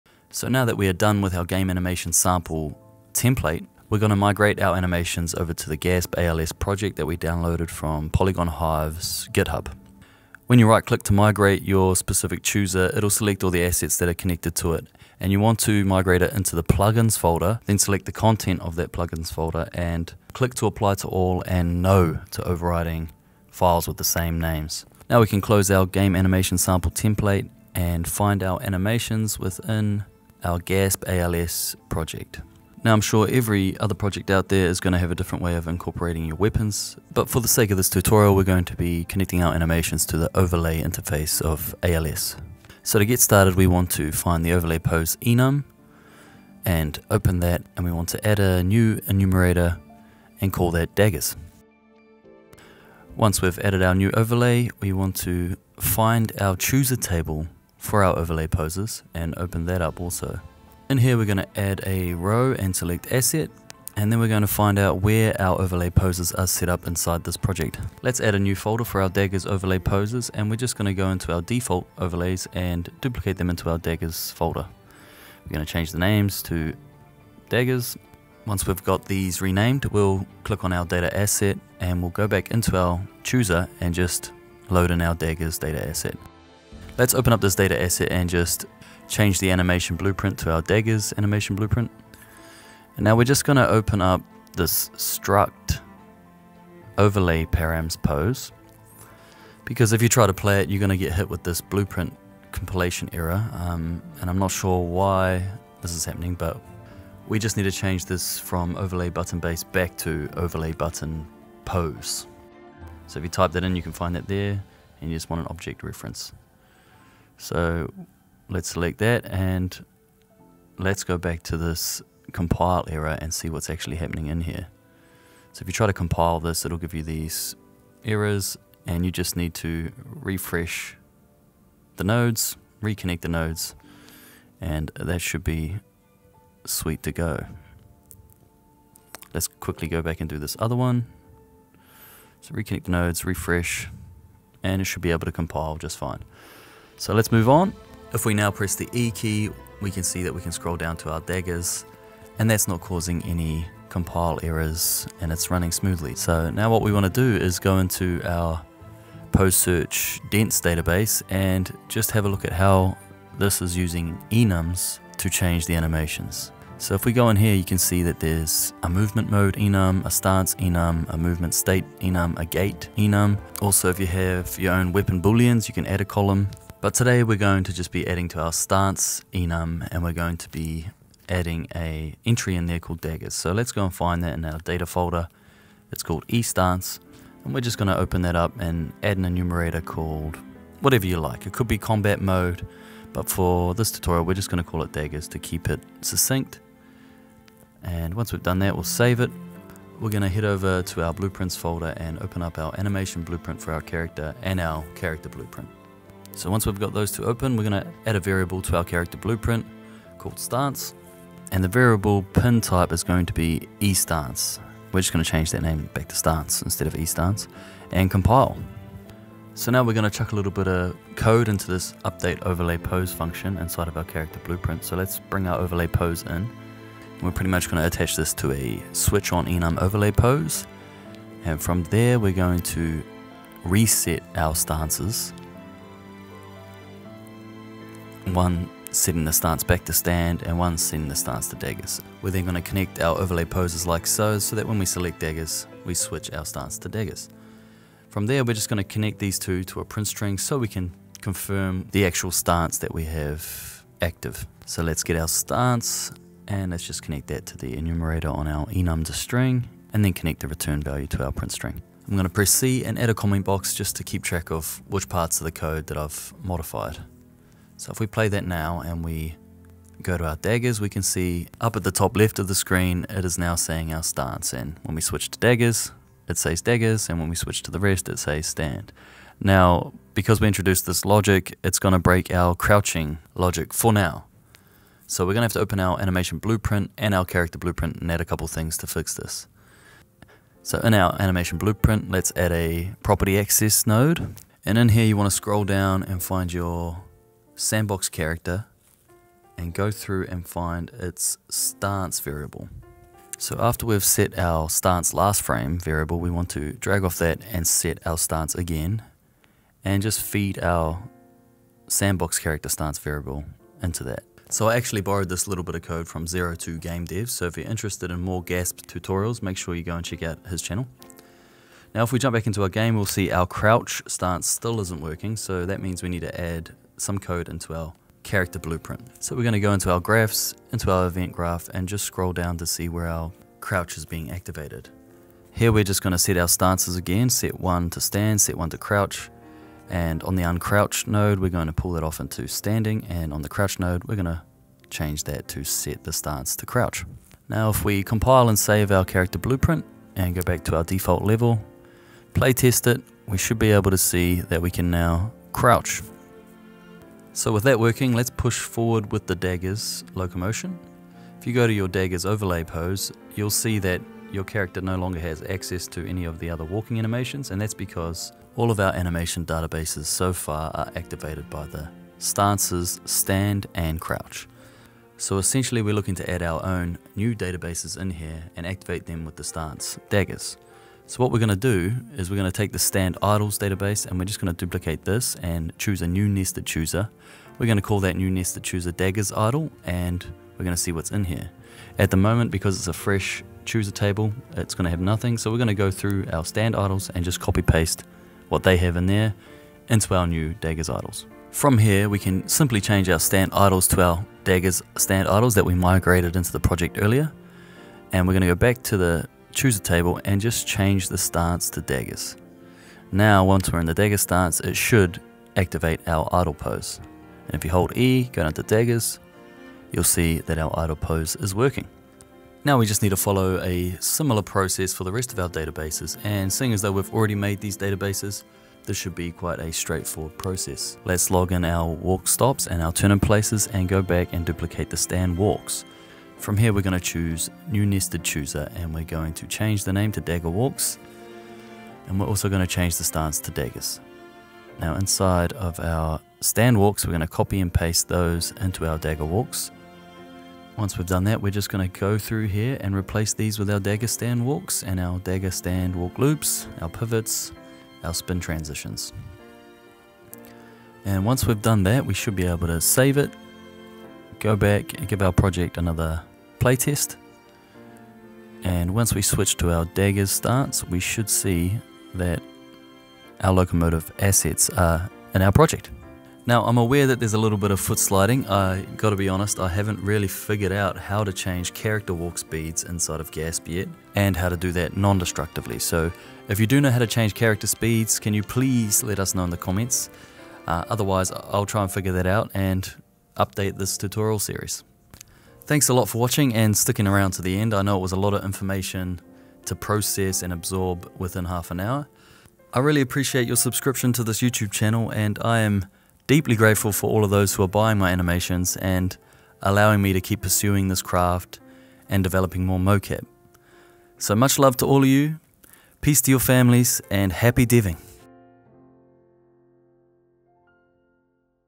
III.I (Pt 09) : Migrating Assets into external projects (GASP + ALSv4 + Enums)
In the final part, we’ll migrate our Dual Daggers locomotion system into the GASPALS project by Polygon Hive:

You’ll learn how to:
- Migrate from your staging project
- Access Pose Search Databases
- Use variables & enums for combat states

This series also acts as documentation for the following locomotion assets on FAB: